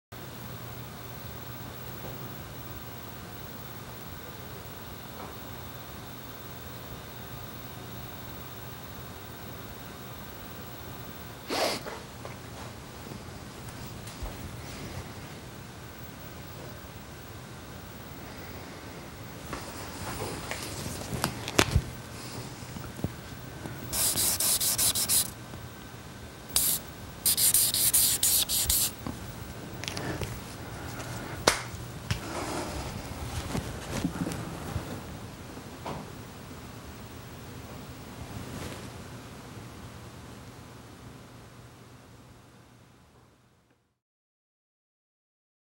Human Dog Lab Entry #104: 4th Amendment to the US Constitution
Originally Posted to Human-Dog.com February 9, 2006.

This video pissed off a home-schooled conservative moron from Kentucky who thought I should keep my focus on fat jokes. I'm guessing Obama has continued these Bush-era violations.